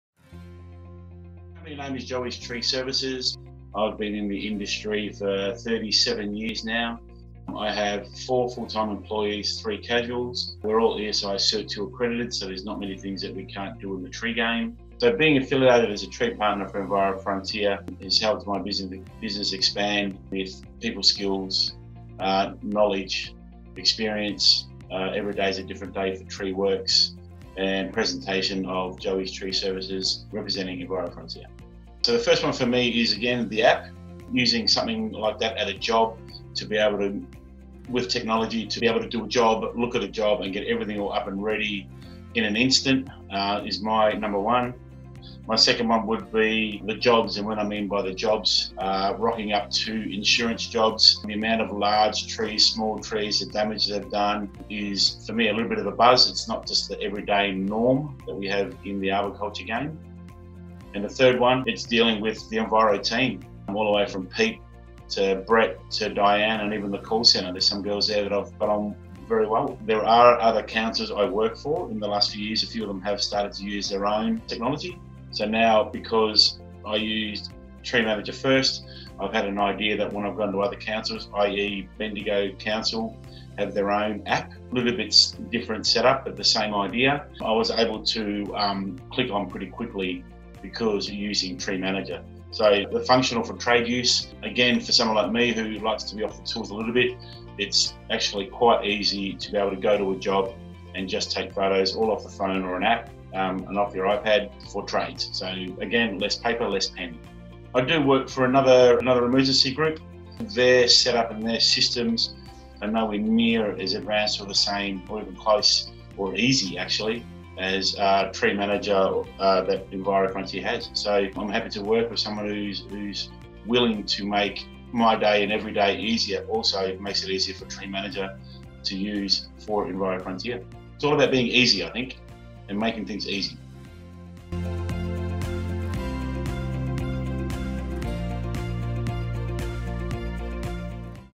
Tree Manager - Joeys Tree Services Testimonial
Joe Castle from Joeys Tree Services shares his views on working with Enviro Frontier